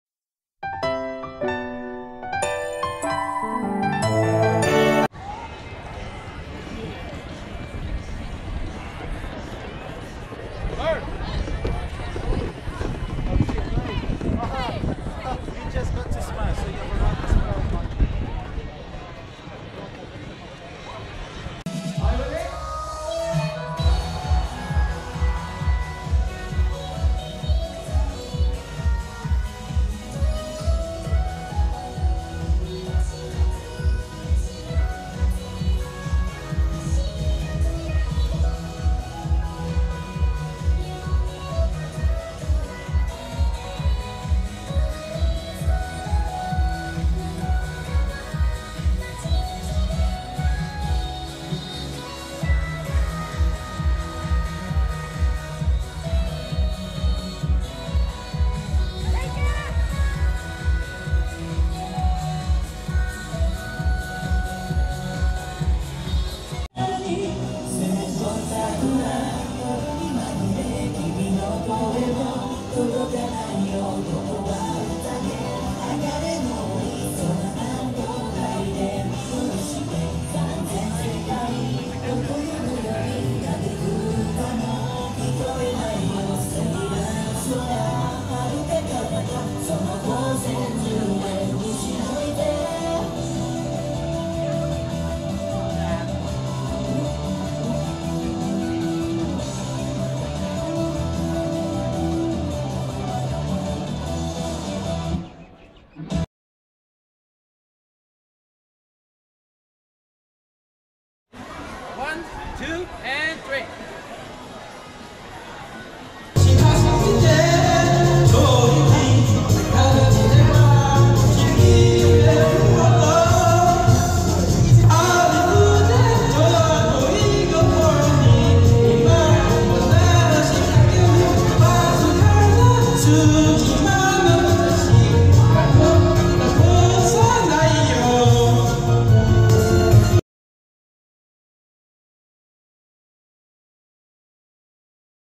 What you may have missed at SMASH! 2016
Here's what you may have missed at SMASH! 2016.

(Video uploaded here because Facebook is being stupid not playing the audio for the video.)